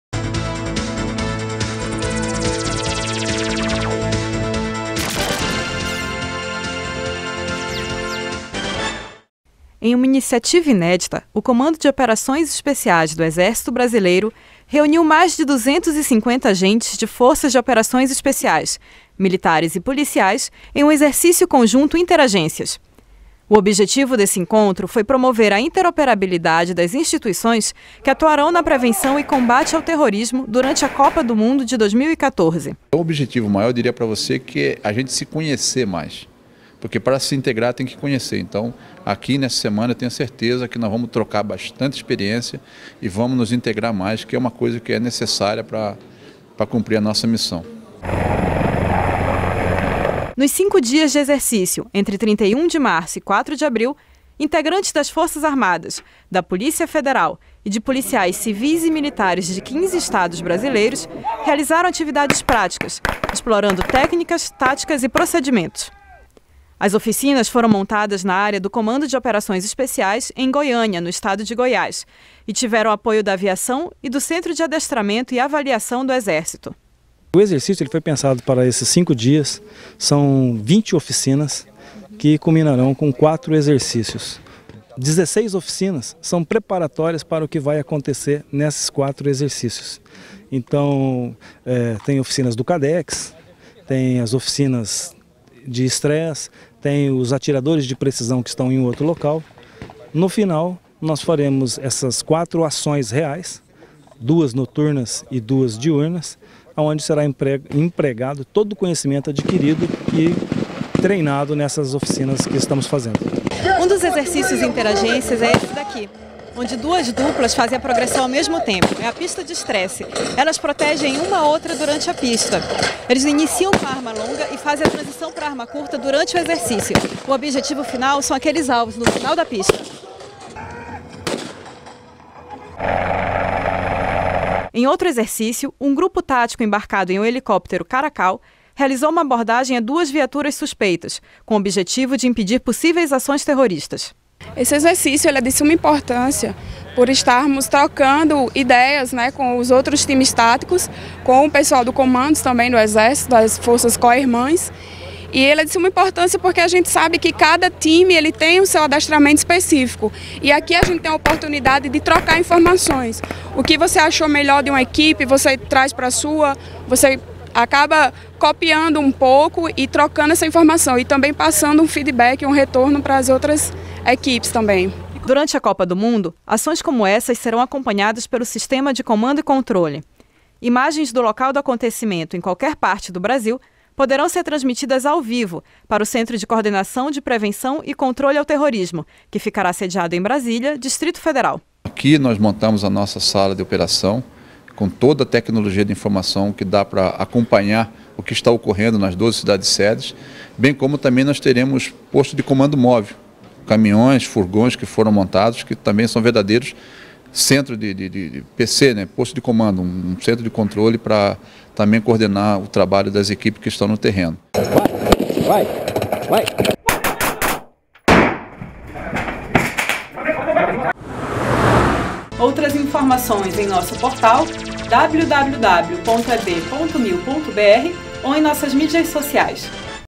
Exercício Interagências no Comando de Operações Especiais
O Exército realizou o Exercício Conjunto Interagências no Comando de Operações Especiais - Goiânia/GO.